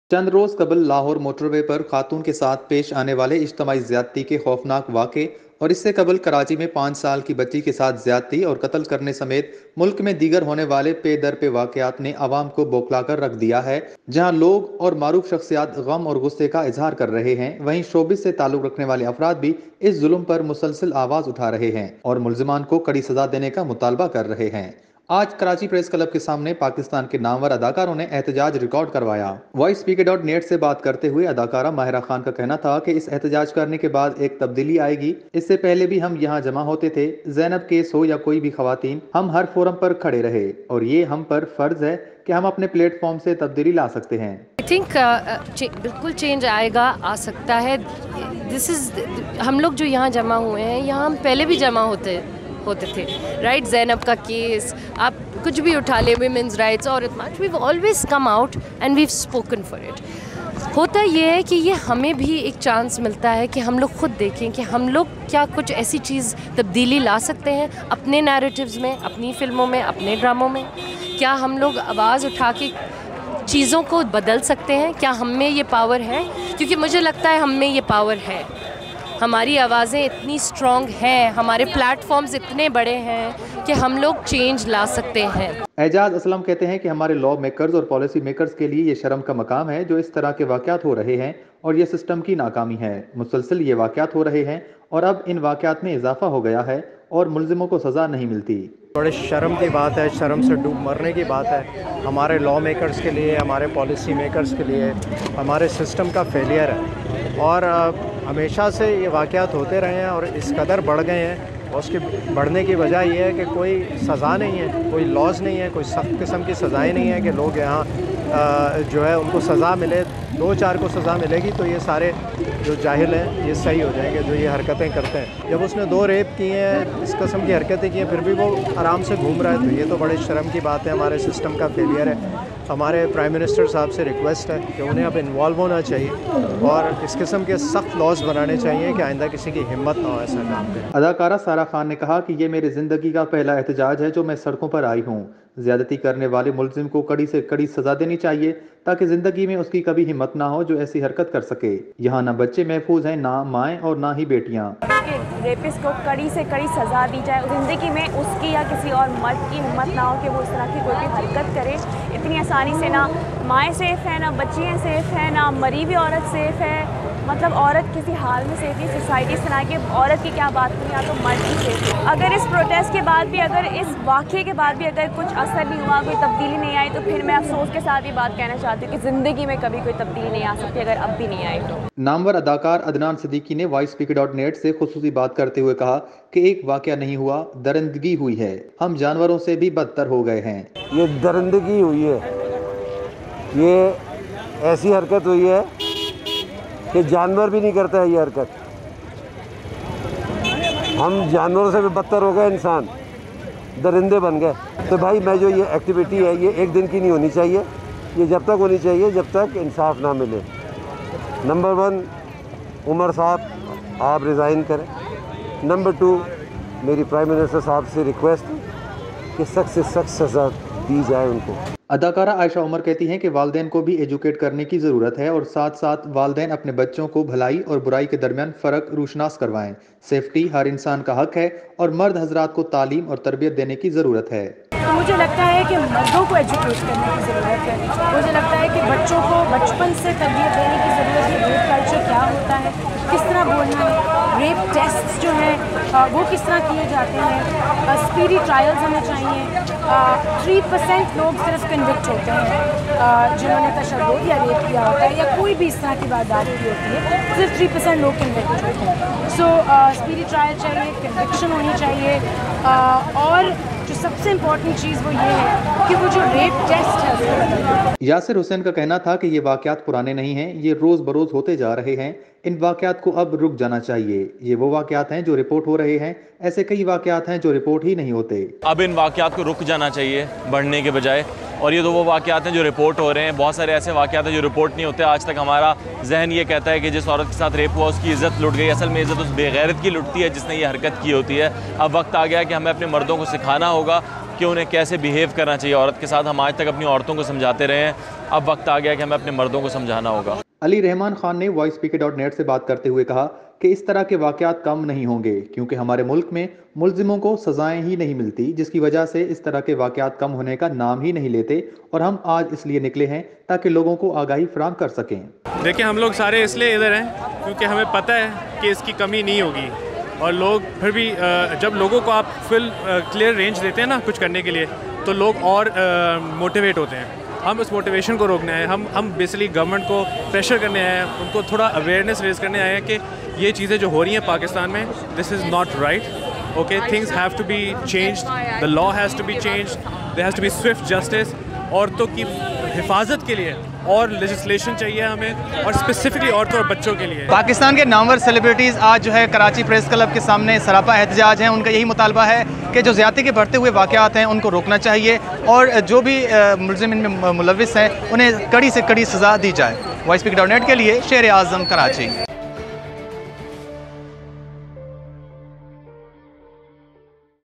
Celebrities can be catalyst of change: Mahira Khan
Celebrities took to demonstrate against Lahore's gangrape case a day later but with full spirit. Actors can act as catalysts and must be vocal about such issues, they say.